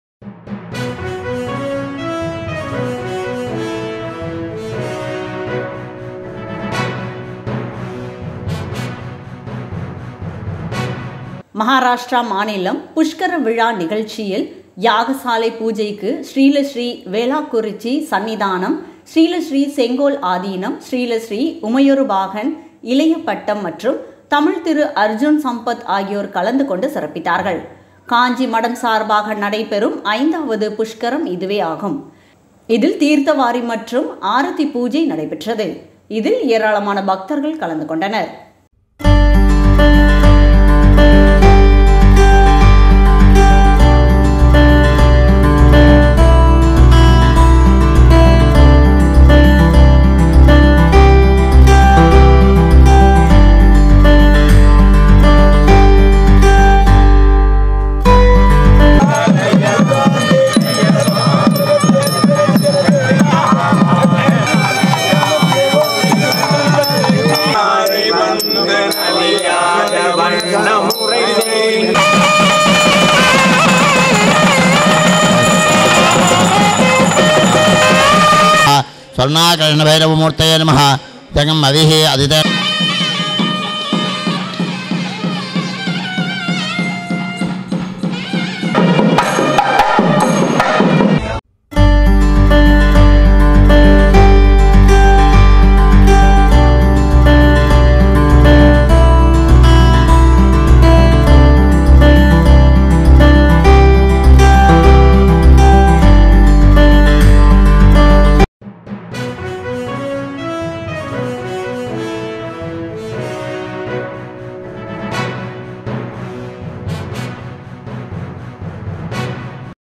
மகாராஷ்டிர மாநிலம் புஷ்கரவிழா நிகழ்ச்சியாக தீர்த்தவாரி மற்றும் ஆரத்தி
மகாராஷ்டிர மாநிலம் புஷ்கரவிழா நிகழ்ச்சியாக யாகசாலை பூஜைக்கு ஸ்ரீலஸ்ரீ வேளாக்குறிச்சி சன்னிதானம், ஸ்ரீலஸ்ரீ செங்கோல் ஆதீனம், ஸ்ரீலஸ்ரீ உமையொரு பாகன் இளைய பட்டம் மற்றும் தமிழ்த்திரு அர்ஜுன் சம்பத் ஆகியோர் கலந்து கொண்டு சிறப்பித்தார்கள் .தீர்த்தவாரி மற்றும் ஆரத்தி கலந்து கொண்ட நிகழ்ச்சி நடைபெற்றது. காஞ்சி மடம் சார்பாக நடைபெறும் ஐந்தாவது புஷ்கரம் ஆகும்.